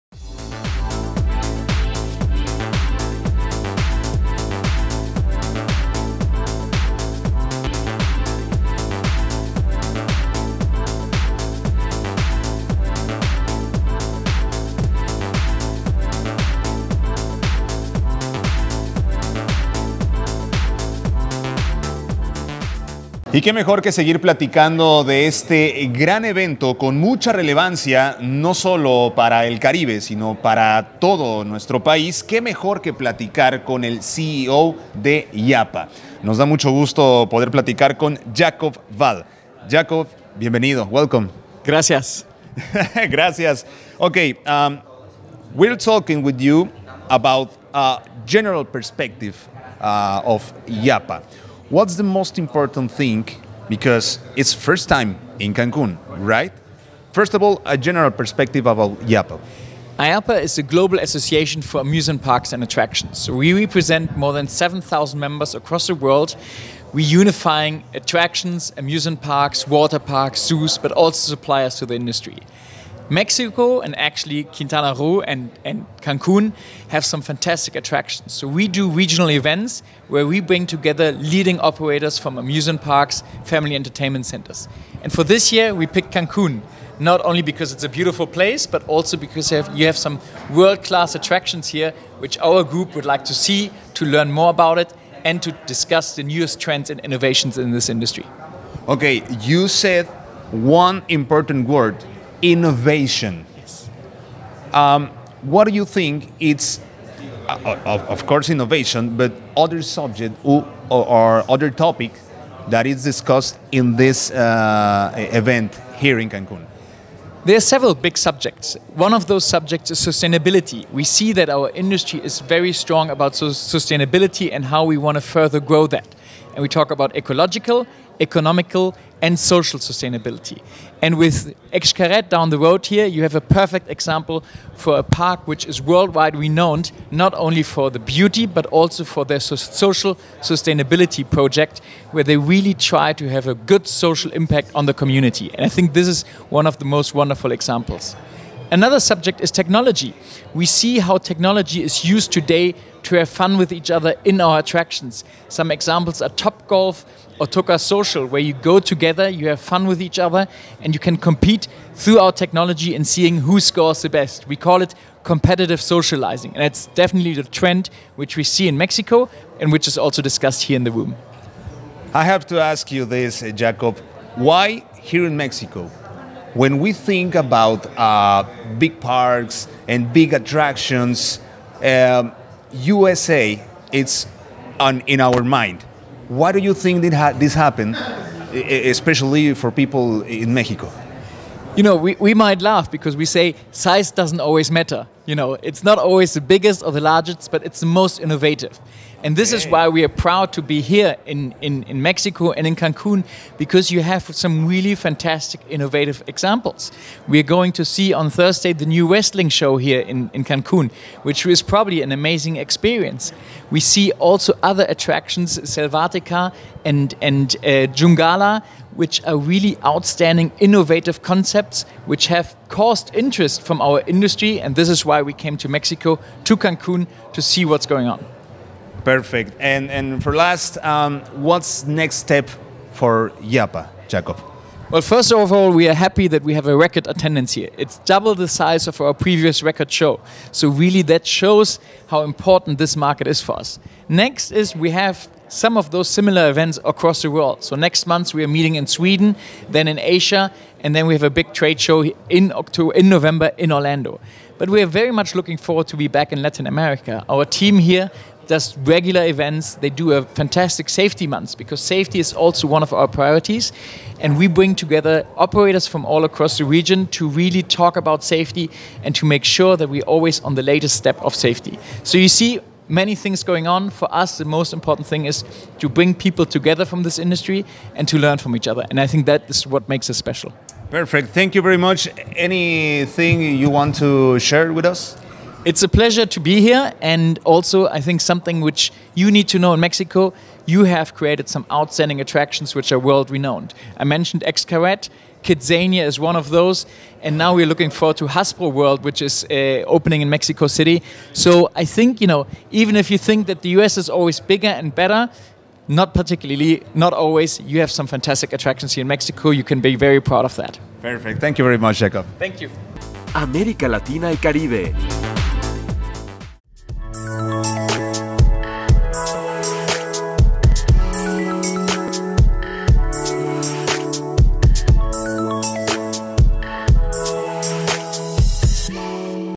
Jakob Wahl, presidente y CEO de IAAPA, durante el encuentro IAAPA
Jakob Wahl, presidente y CEO de IAAPA, durante el encuentro IAAPA: América latina y Caribe, llevado a cabo del 24 al 27 de abril del 2023 en Cancún y Riviera Maya, México.